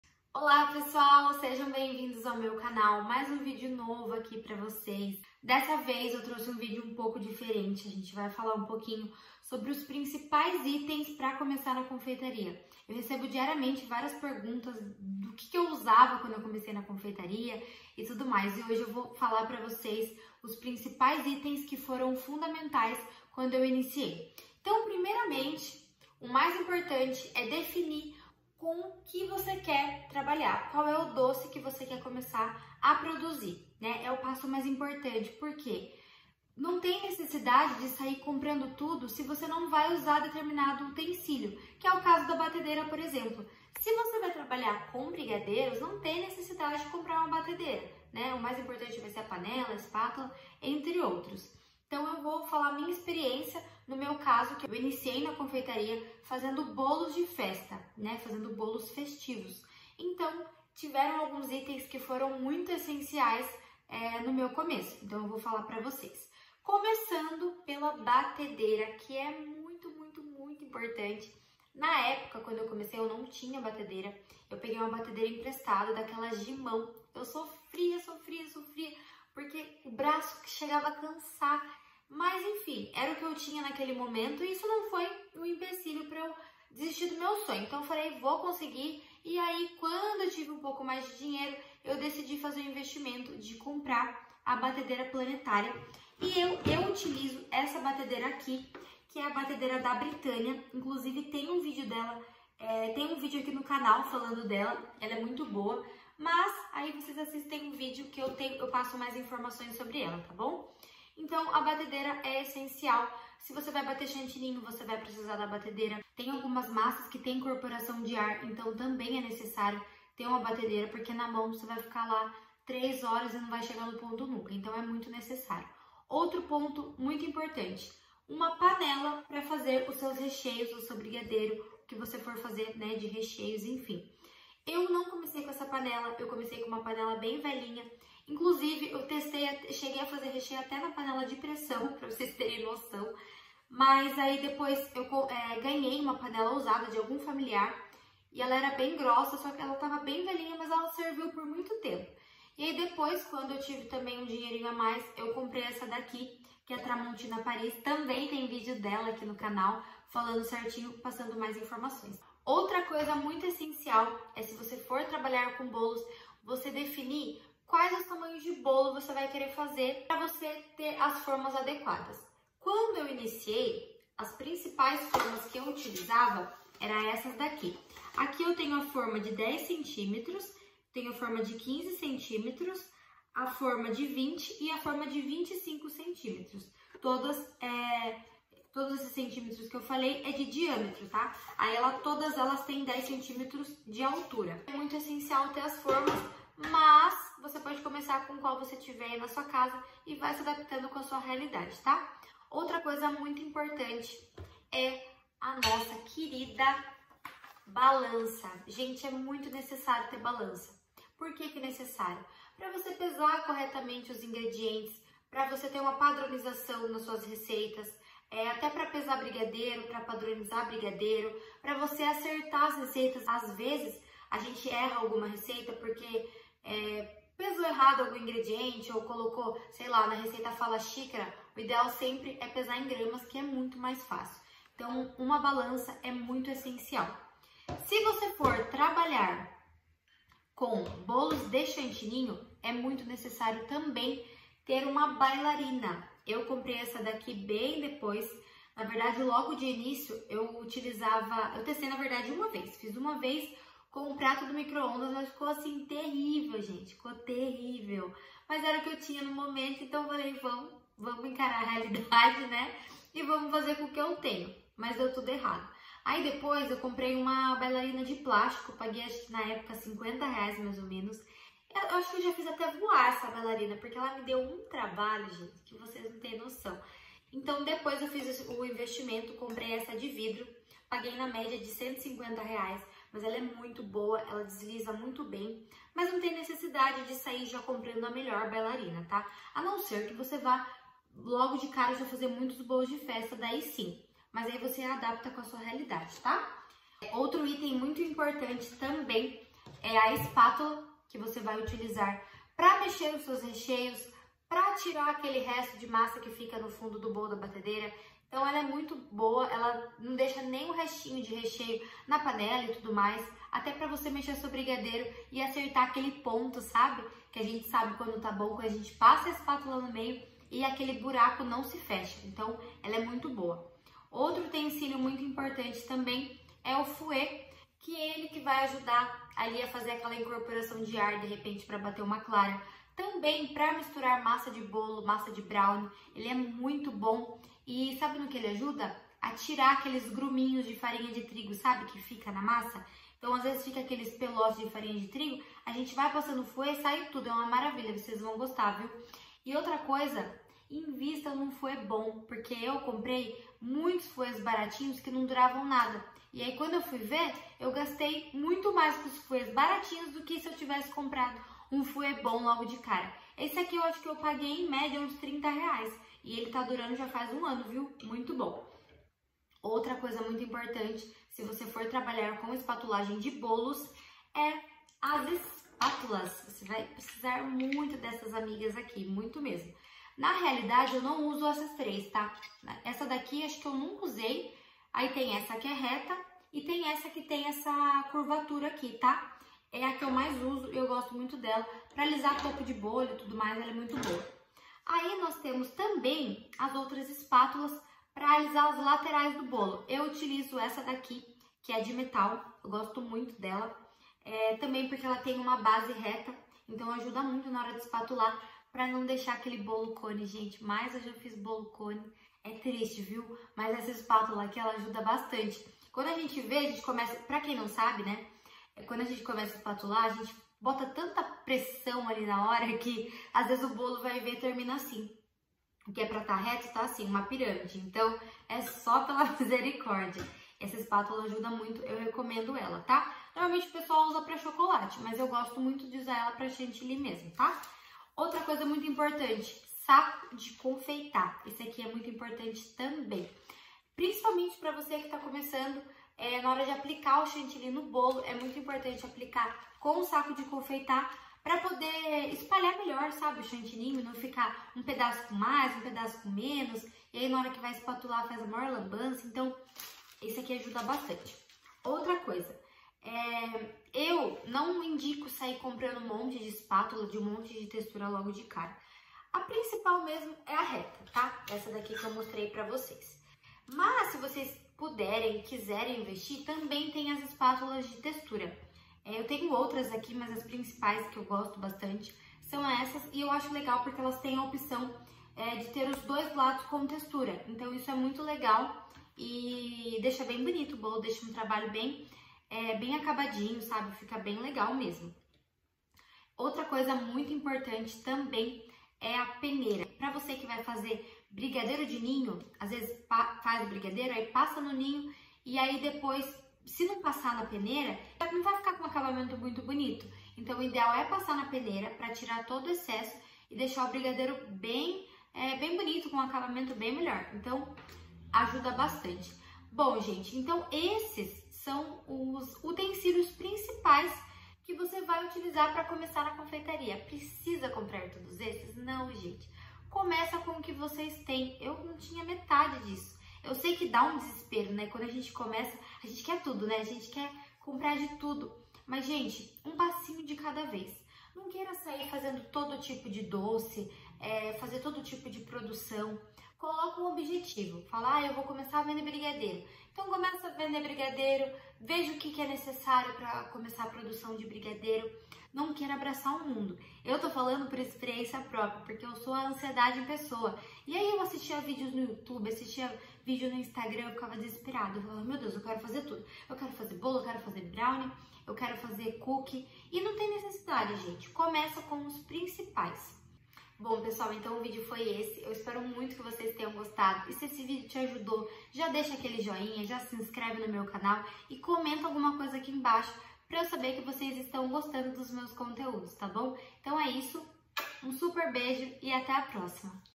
O QUE EU PRECISO PARA COMEÇAR A VENDER DOCES - UTENSÍLIOS ESSENCIAIS
Olá pessoal ❤️❤️❤️🫶

Hoje trouxe um vídeo um pouco diferente ❤️

Espero ajudar vocês que estão pensando em começar na confeitaria, qualquer dúvida, estou a disposição 🫶